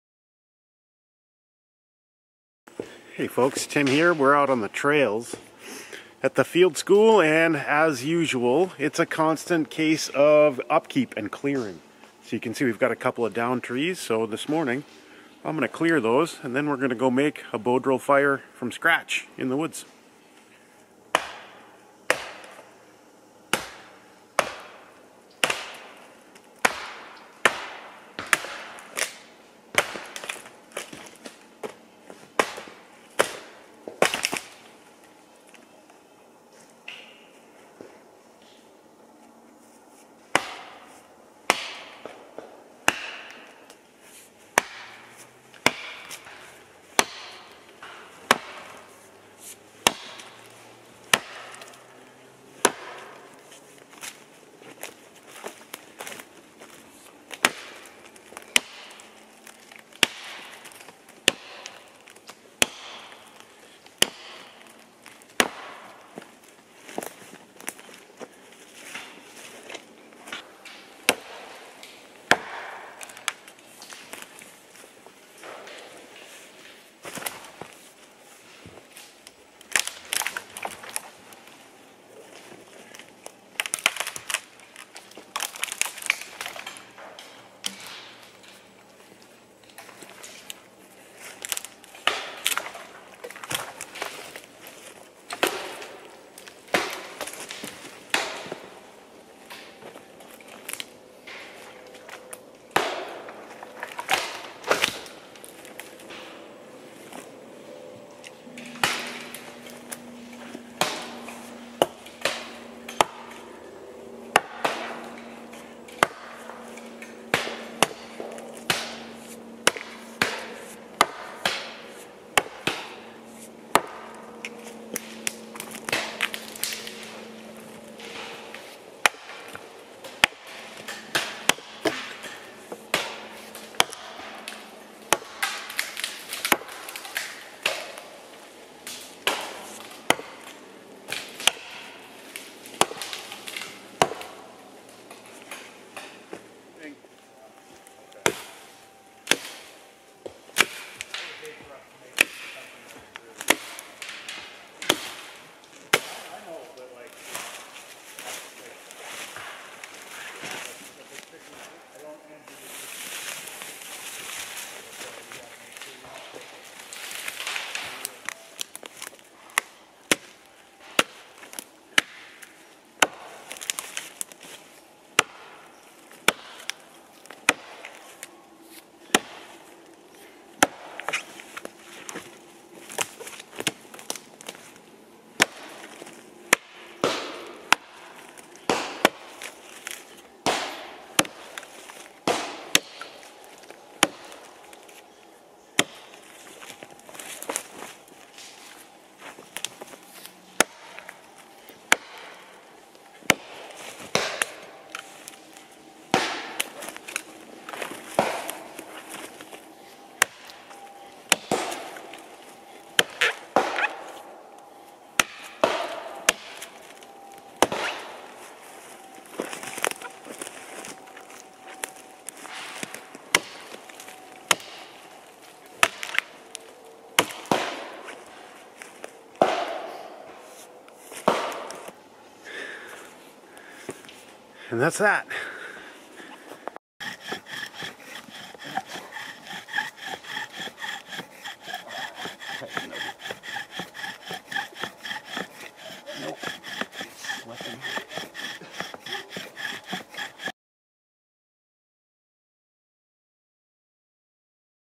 JMB Vlog 146 | Axe Work And Trail Maintenance.mov
Keeping the trails at the field school open is an ongoing process. Here we were out on a crisp October morning clearing downed trees with axes.